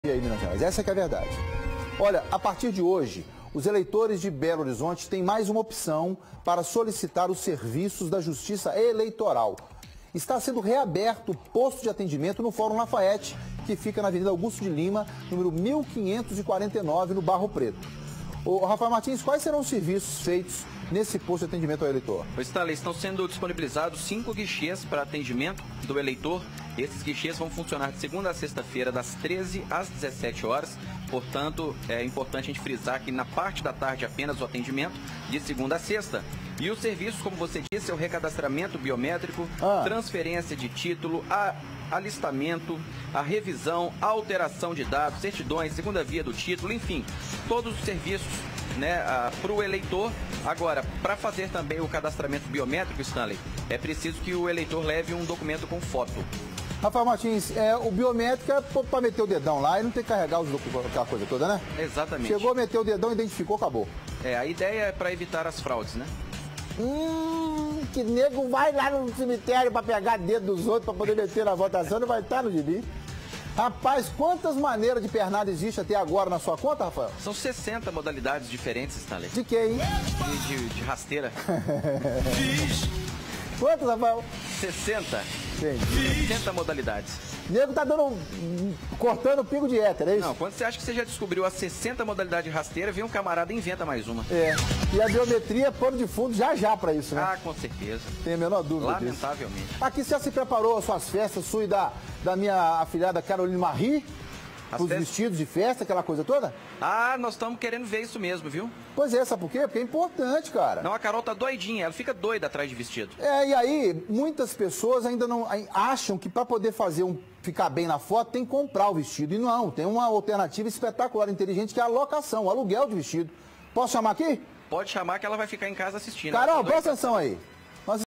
Atendimento ao eleitor é reaberto no Fórum Lafayette
Os eleitores da capital tem mais uma opção para solicitar os serviços da Justiça Eleitoral.